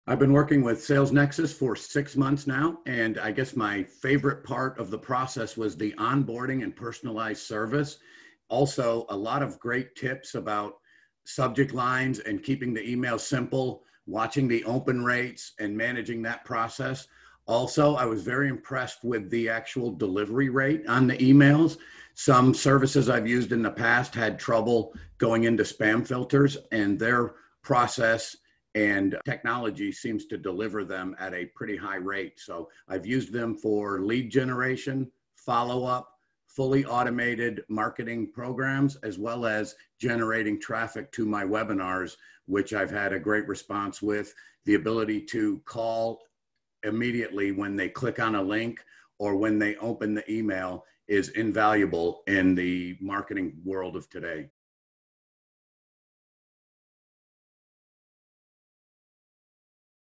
How Mike Colesante generated leads with the SalesNexus Email Marketing Service
Mike Colesante of Backup Technology talks about how the SalesNexus Email Marketing Service has helped him generate leads, increase webinar registrations and reach new potential customers.
Mike describes how the Email Marketing Service has improved the quality and deliverability of his email campaigns.
The SalesNexus Email Marketing Service is a full service email campaign creation and management package available to SalesNexus customers